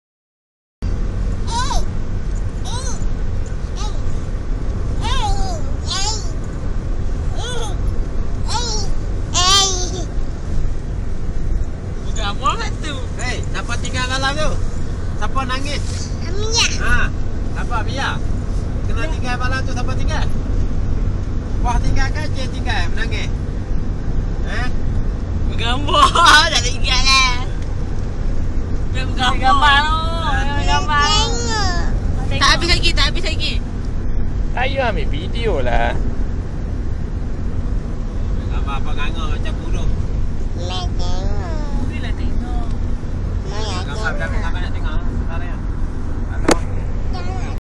Mira suka melawak dengan Mak Su Aishah
Di dalam kereta dalam perjalanan ke Selama.